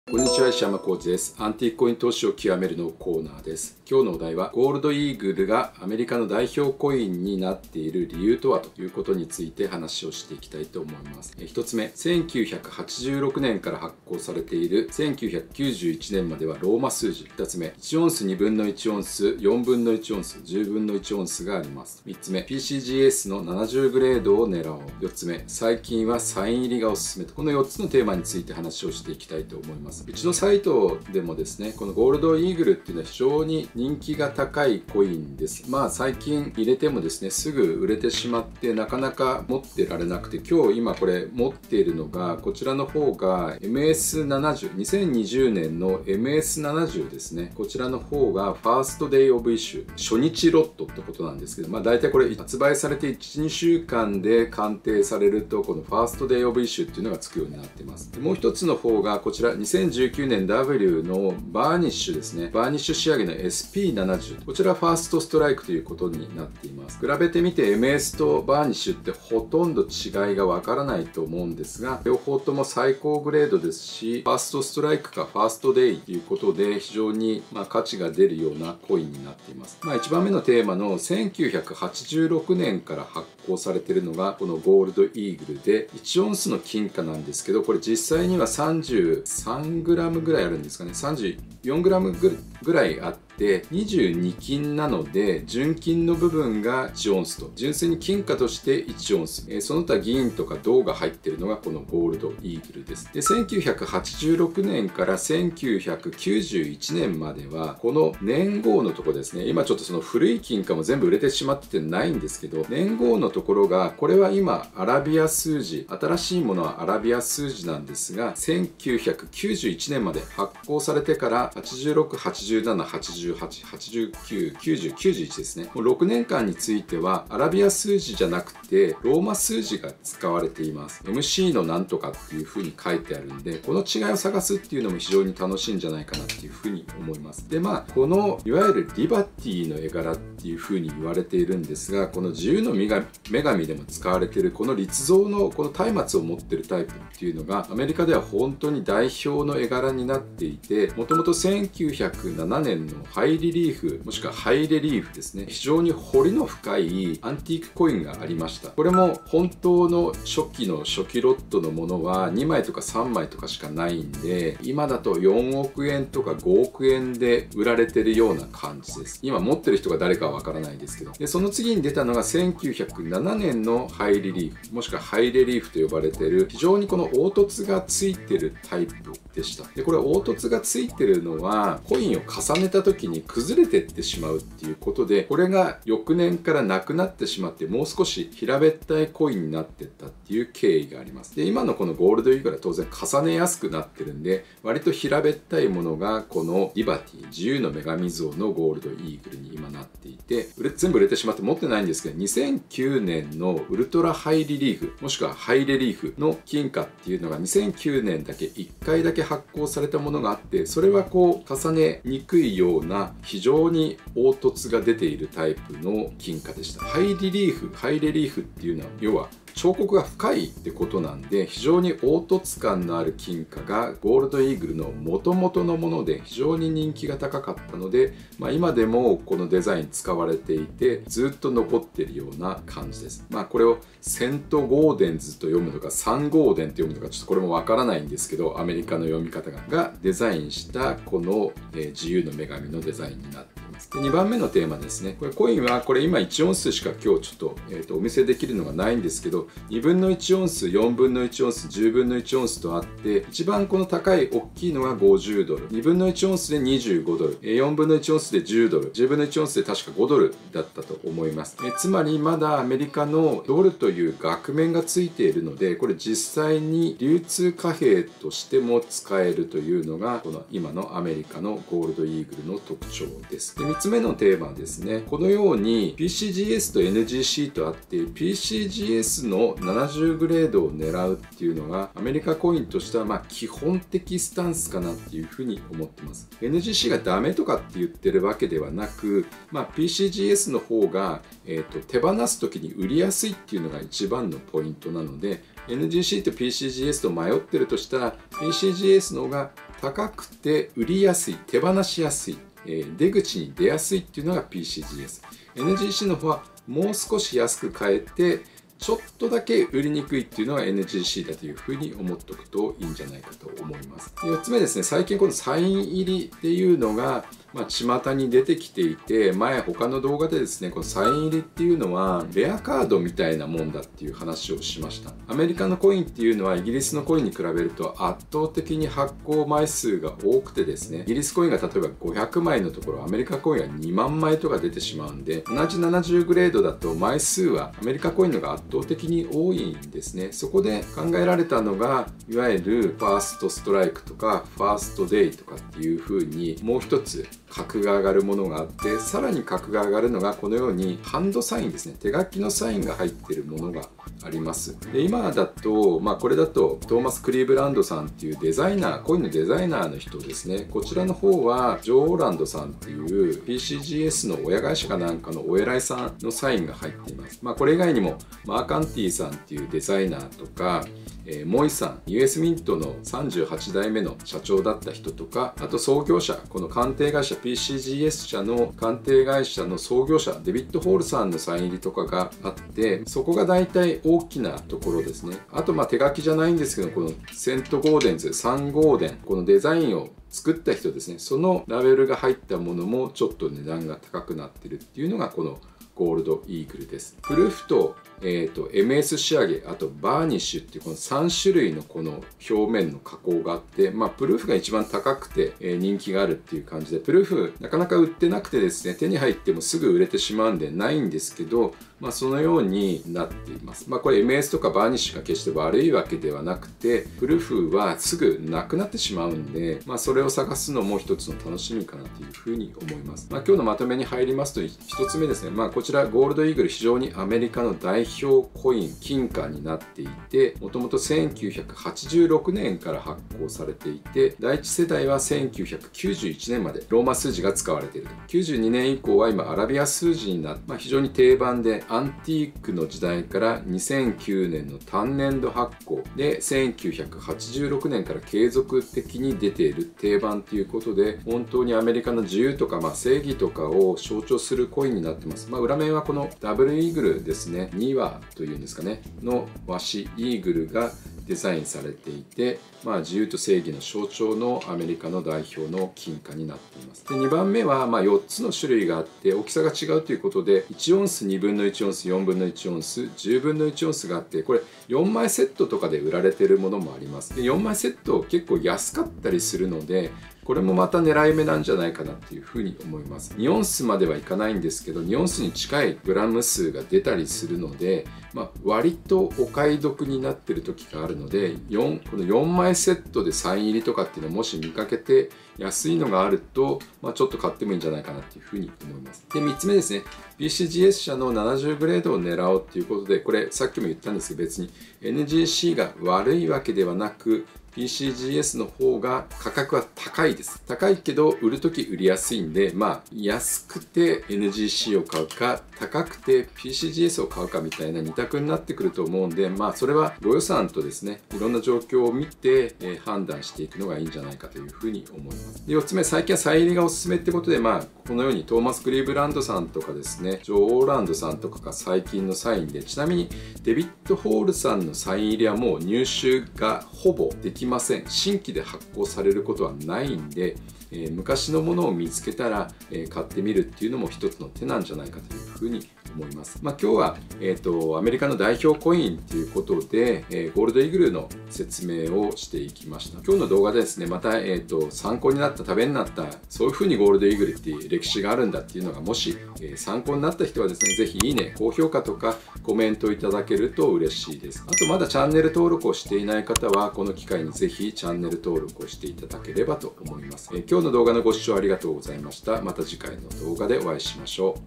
アンティークコインやモダンコインを学ぼう② ゴールドイーグルがアメリカの代表コインになっている理由とは？
金貨販売のゴールドコインでは、モダン金貨、アンティークコインを取扱います。

【ZOOM対応】曜日、時間指定可能　個人向けアンティークコインセミナー